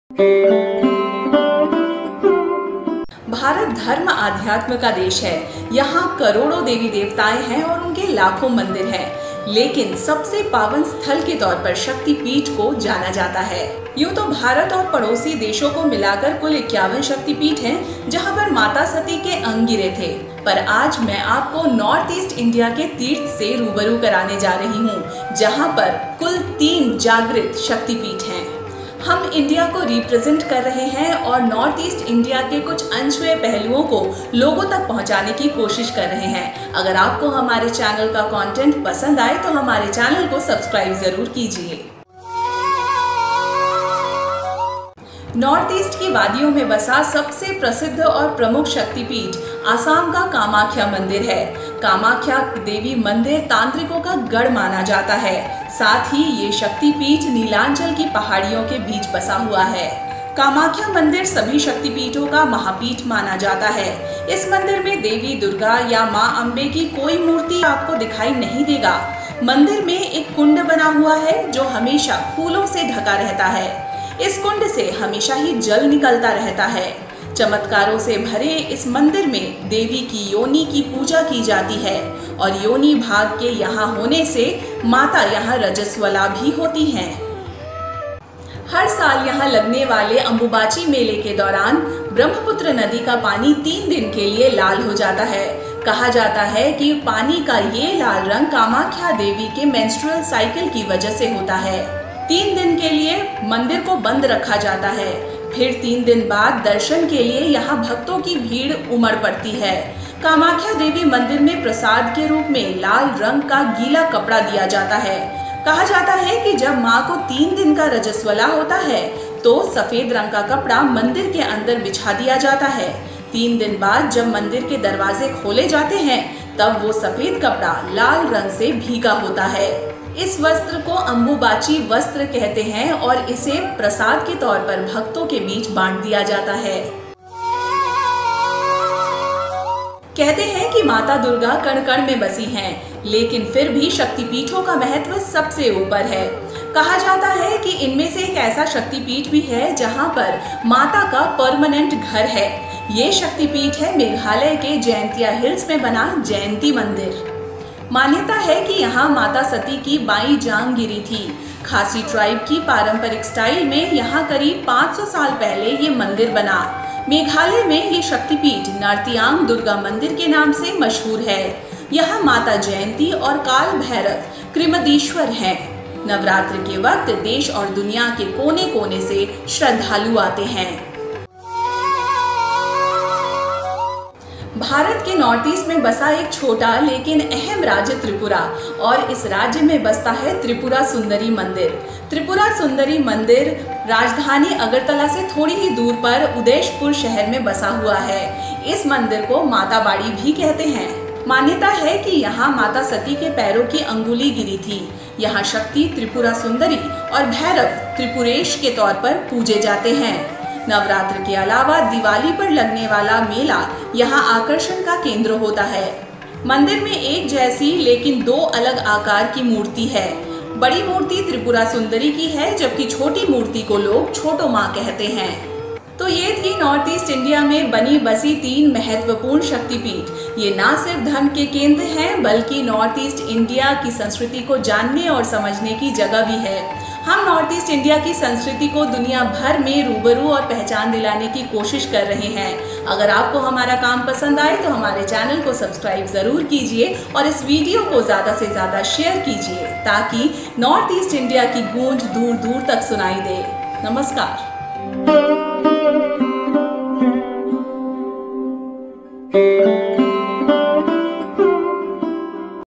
Fascinating and Famous Shaktipeeths of North East India which you must visit in your lifetime
North East India better known for its diverse culture and customs, is also the home of 3 most significant Shaktipeeths. The importance of the Shaktipeeths is well mentioned in the religious books and purans. Scattered in Indian Sub-continent, these 51 Shaktipeeths are considered to be the most sacred place among all.
Hinduism is spread world wide but many people would not know that Out of the 51 shaktipeeths in hindu dharm,  there are major three very significant shaktipeeths present in north east India. On the occasion of Navratri and Durga Puja , we are trying to re-present the religious and customary importance of these Shaktipeeths.

# These are the links of other interesting videos on NorthEast India which you may like 👇

# Your suggestions are valuable so that I can keep on improving our videos.